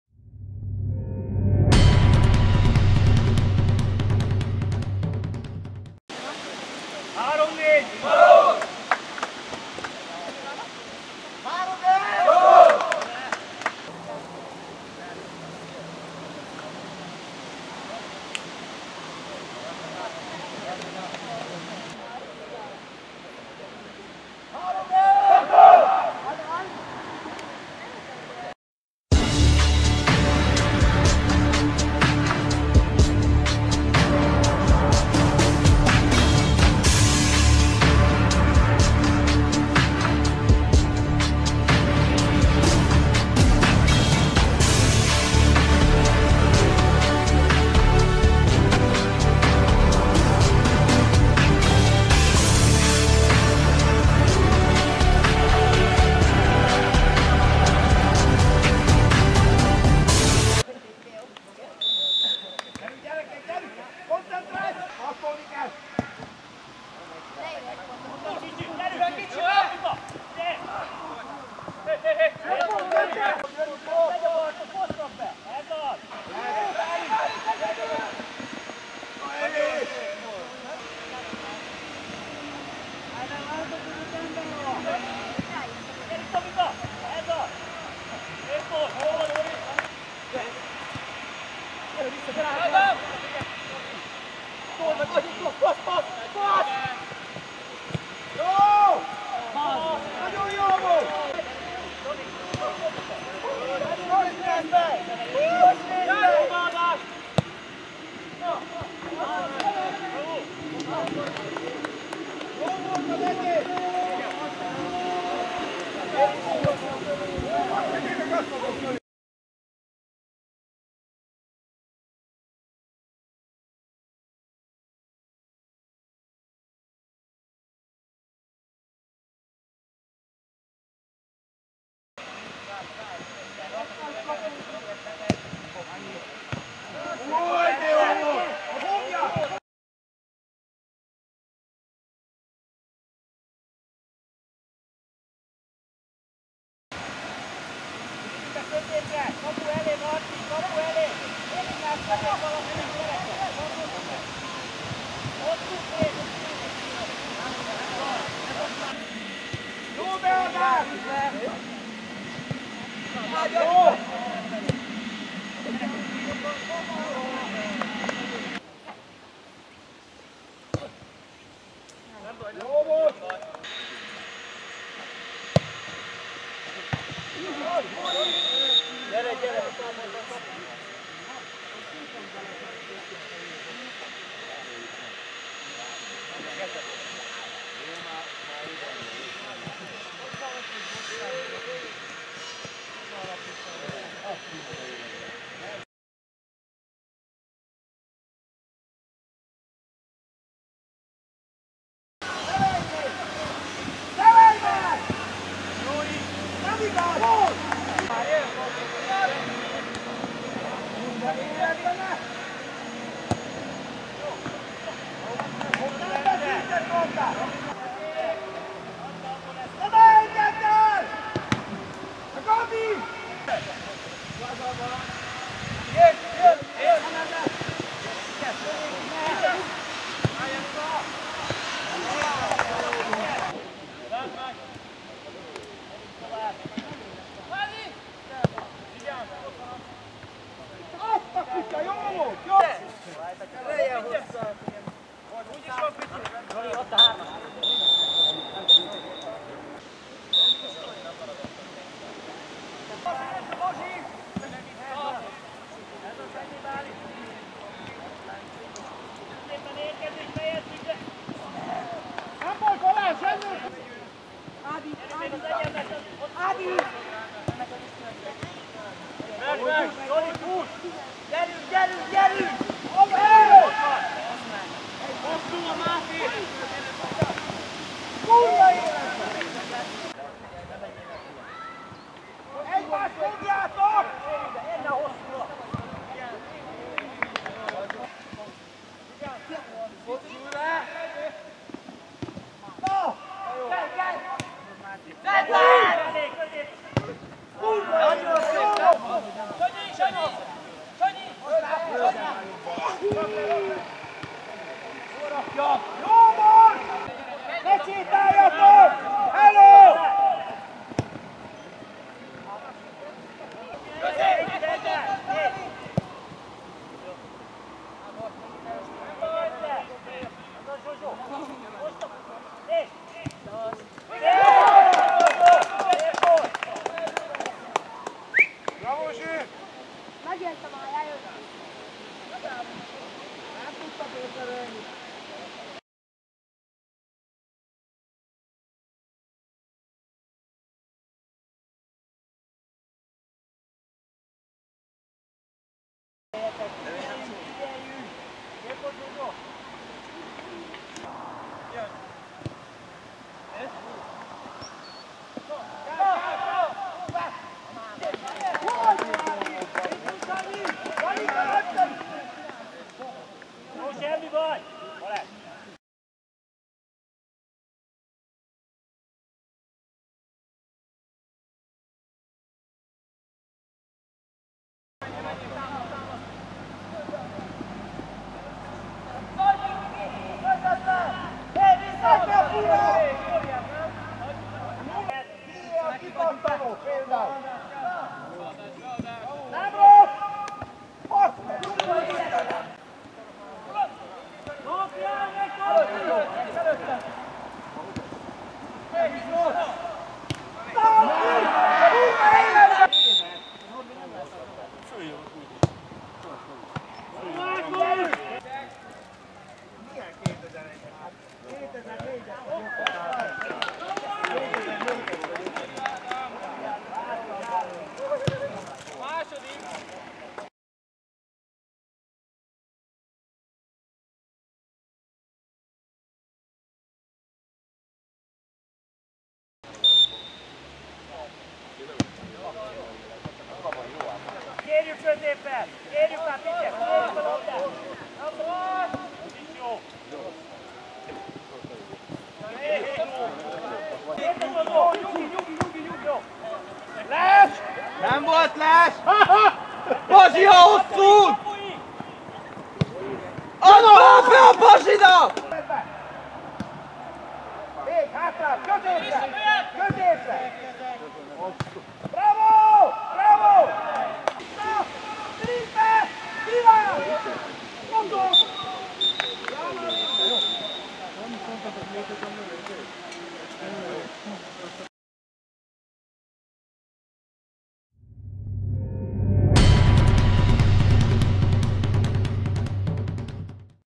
Tokod SE U19 – Pilismarót SC U19 1-3 (0-1) - Összefoglaló
M.II.O. U-19 ÉSZAK 7. forduló
Vezette: Szalay Norbert (Áy János Hunor)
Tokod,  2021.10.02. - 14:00

0-1 17' Sárközi Ádám
1-1  61' Boros Zsolt
1-2 64' Sárközi Ádám
1-3 73' Sárközi Ádám